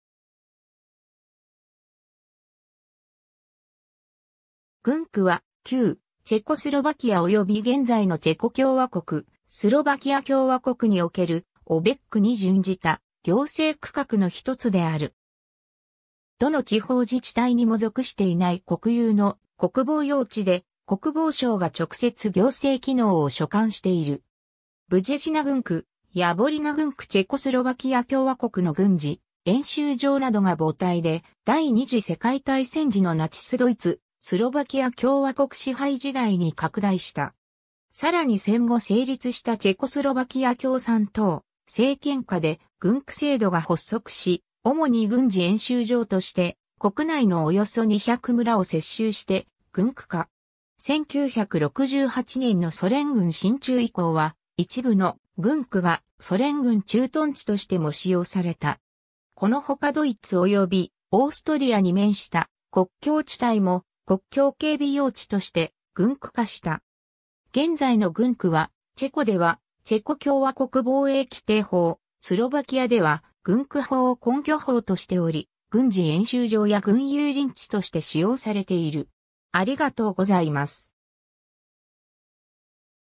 軍区 (チェコ、スロバキア)
軍区（チェコ語: Vojenský újezd, チェコ語発音: [ˈvojɛnskiː ˈuːjɛst], スロバキア語: Vojenský obvod, 英語: Military district）は、旧チェコスロバキアおよび現在のチェコ共和国、スロバキア共和国におけるObec（村・基礎自治体）に準じた行政区画の一つである。
どの地方自治体にも属していない国有の国防用地で、国防省が直接行政機能を所管している。
 ブジェジナ軍区（チェコ・南モラヴィア県） ヤヴォリナ軍区（スロバキア・プレショフ県） チェコスロバキア共和国（第一・第二）の軍事演習場などが母体で、第二次世界大戦時のナチスドイツ、スロバキア共和国（第一）支配時代に拡大した。
さらに戦後成立したチェコスロバキア共産党政権下で軍区制度が発足（1949年法律第169号）し、主に軍事演習場として国内のおよそ200村を接収して軍区化。
1968年のソ連軍進駐以降は、一部の軍区がソ連軍駐屯地としても使用された。
このほかドイツおよびオーストリアに面した国境地帯も国境警備用地として軍区化した。
 現在の軍区は、チェコではチェコ共和国防衛規定法（チェコ共和国議会1999年法律第222号）、スロバキアでは軍区法（スロバキア共和国国民議会1997年法律第281号）を根拠法としており、軍事演習場や軍有林地として使用されている。